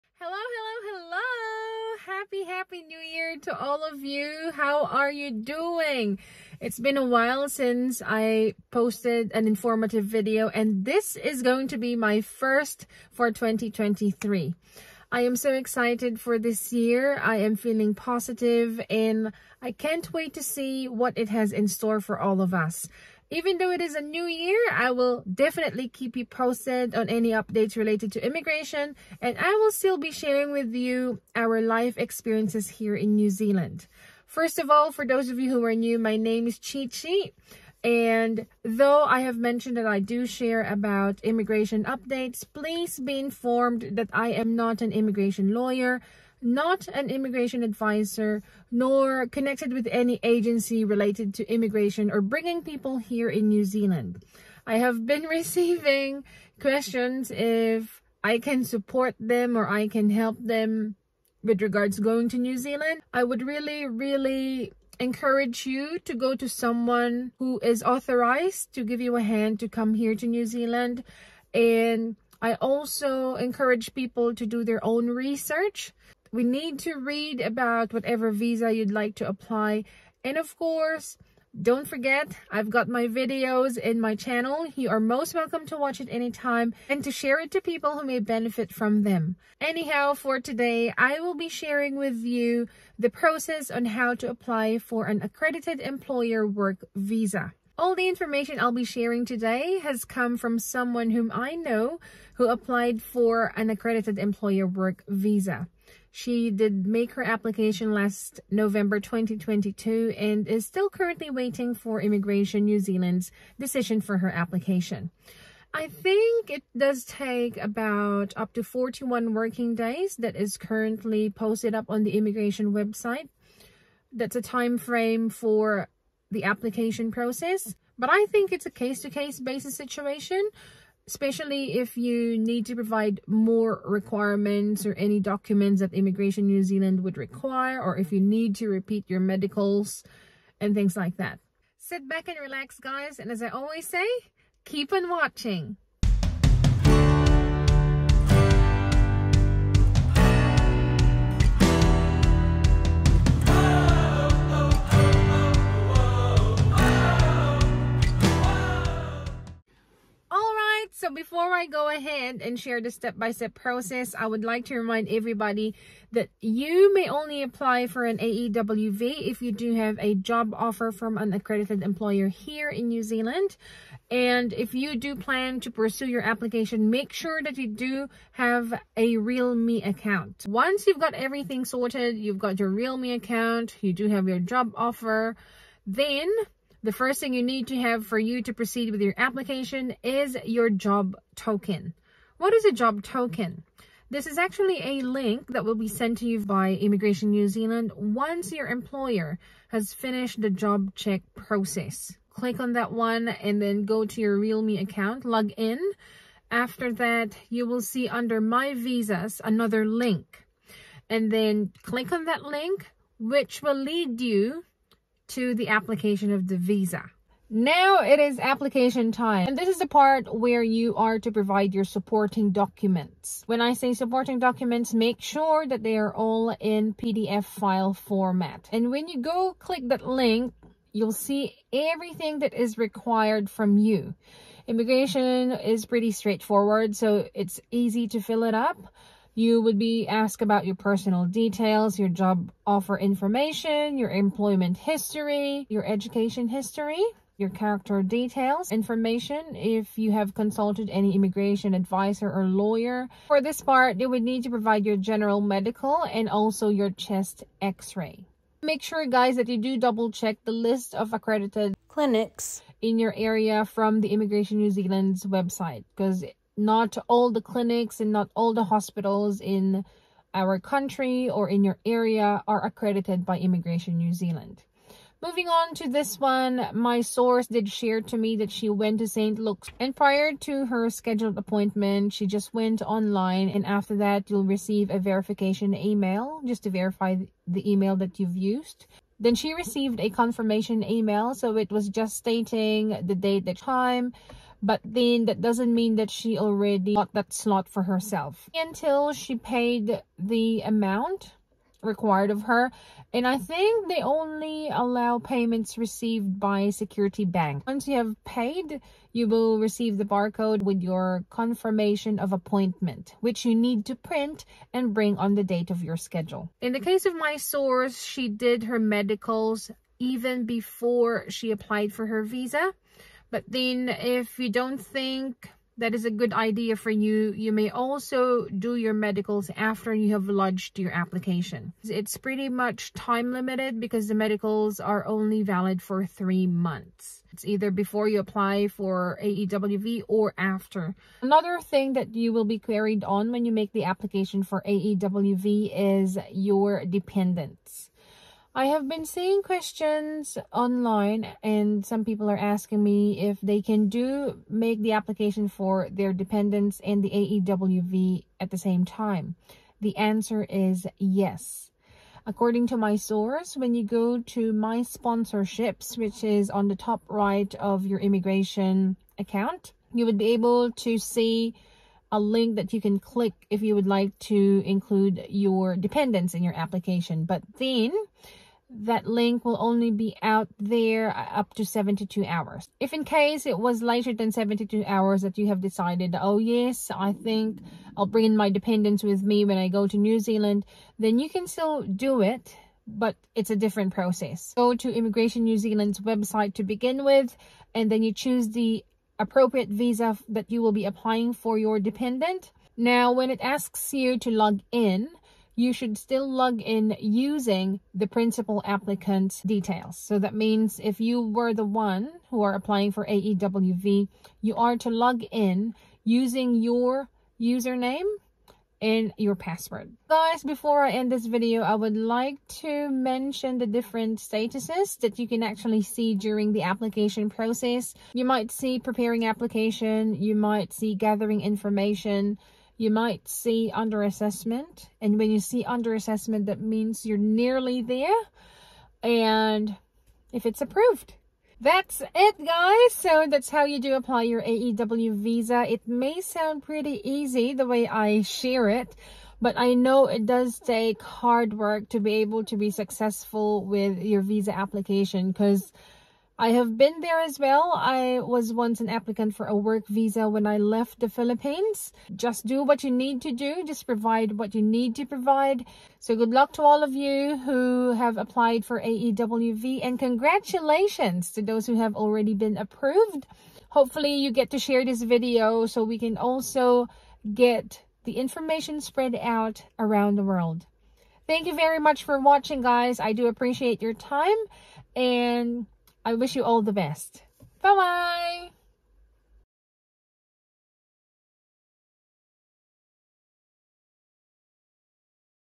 Chi Chi Shares: Straight from an Applicant!!! AEWV Application Process ||ChiChiOnRecord
This video provides the viewers information on how to apply for an ACCREDITED EMPLOYER WORK VISA.
Information gathered was supplied by an AEWV applicant based on her personal experience, which I am sharing with you all.
Hope you learn from today's vlog and please share! :)

I am not connected with any Immigration nor recruitment agencies.

New Zealand Visitor Visa Vlog

Accredited Employer Work Visa Vlog (Updated)

Bringing partners to NZ with an AEWV